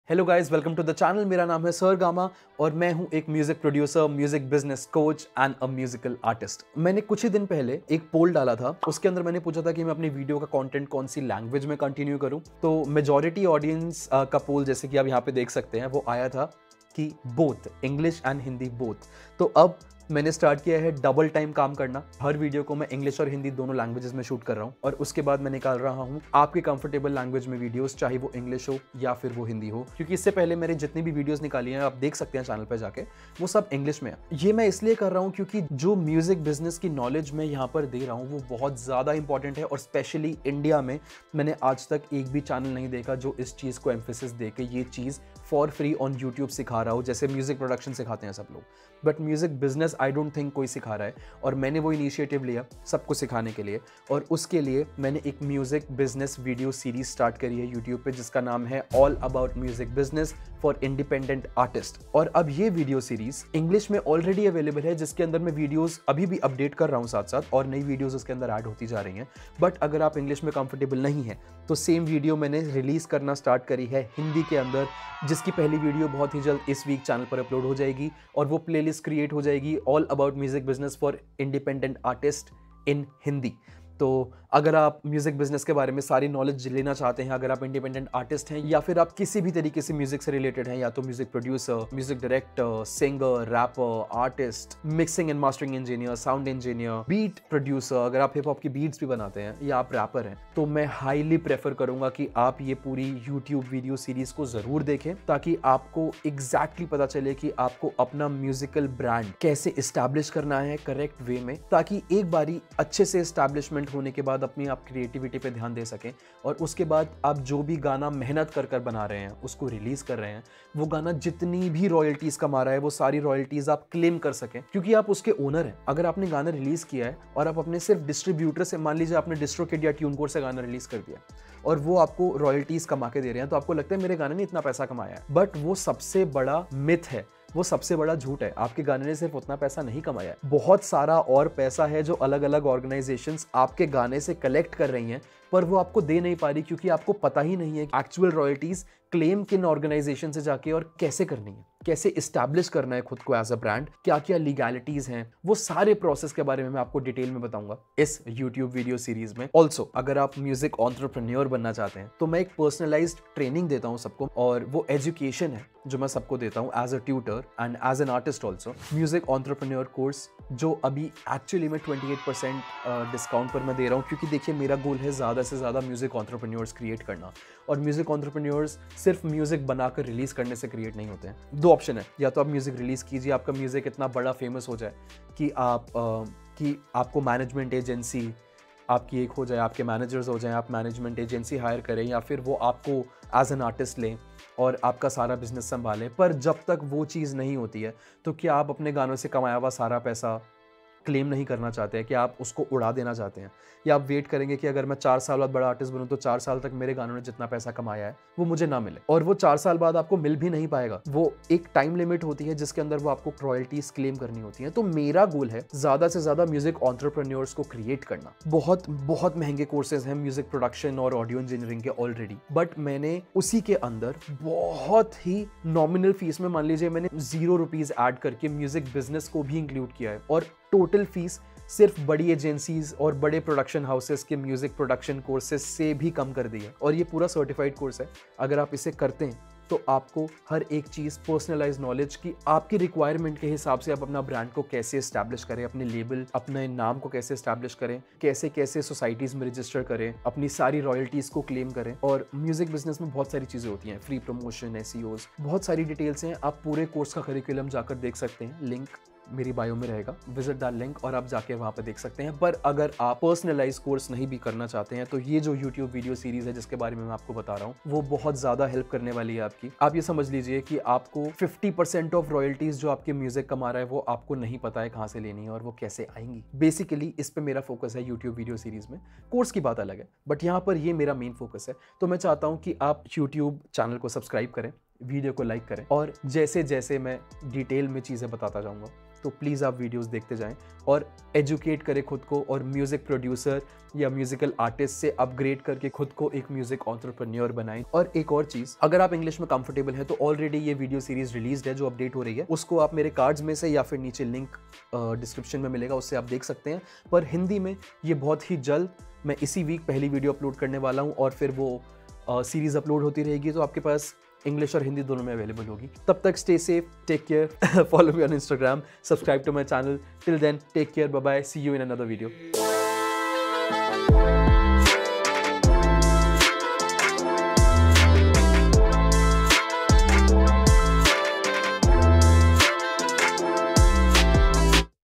All About Music Business for Independent Artists Now in HINDI
Learn Music Business, Music production, Audio Engineering one on one with me or call (only for course enquiries) on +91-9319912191

As per the poll I put out on Instagram, now Music Business for Independent Artists is also available in HINDI!

2. Music Production Audio Engineering